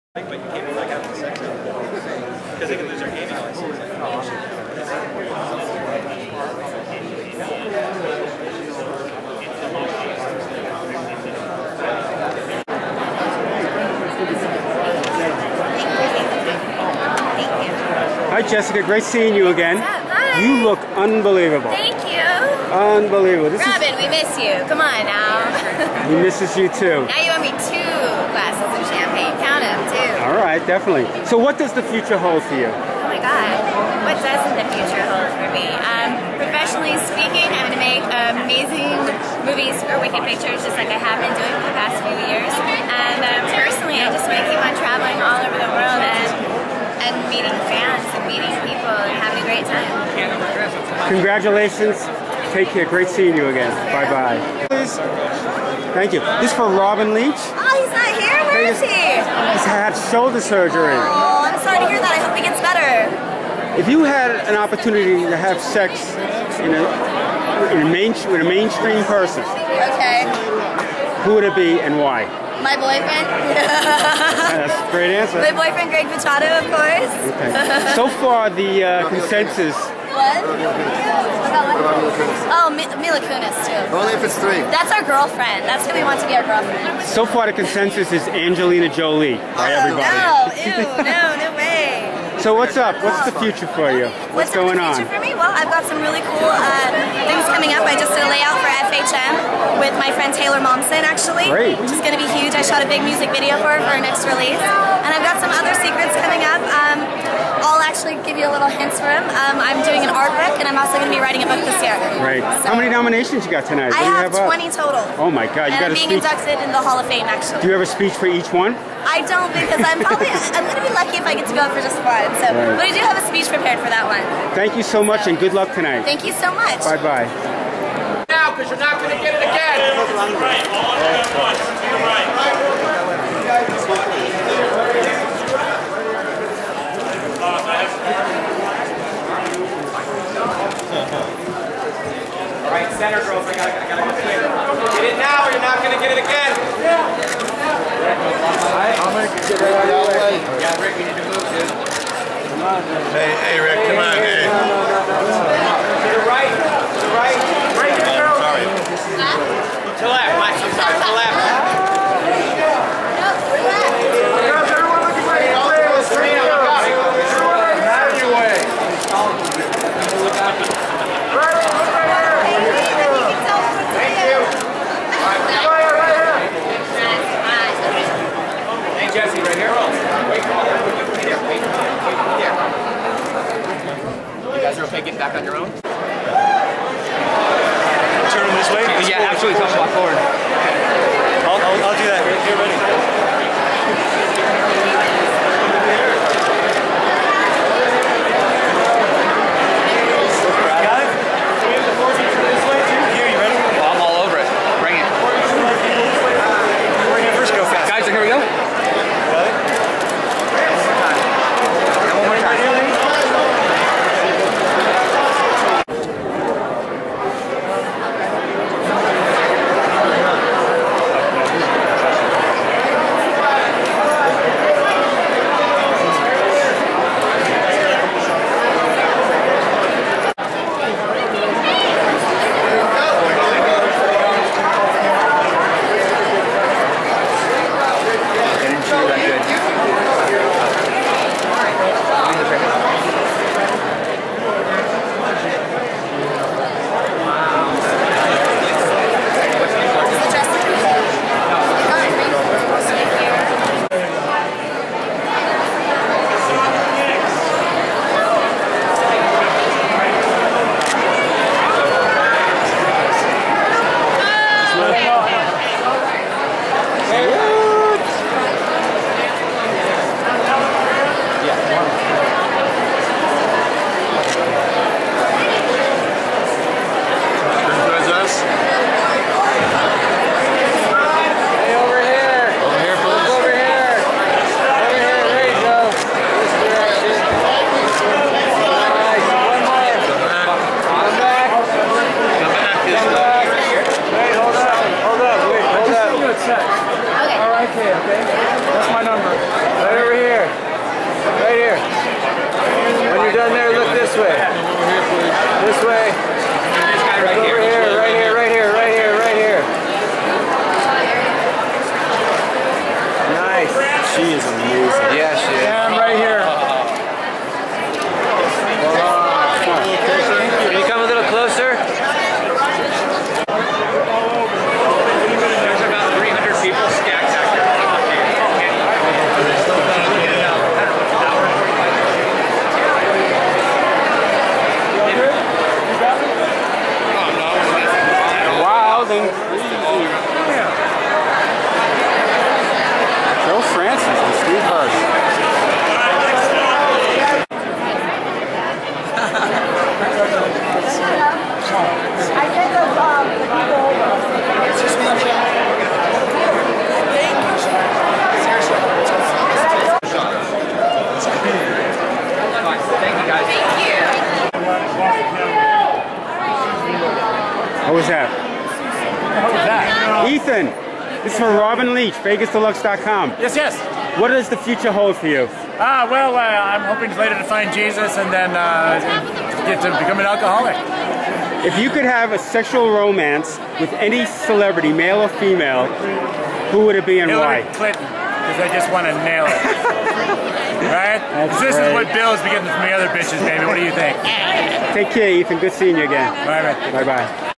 2012 AVN AWARDS show red carpet interviews and atmosphere
Jesse Jane, Kayden Kross, Riley Steele, Bibi Jones, Jessica Drake, Jenna Haze, Kagney Karter. read the story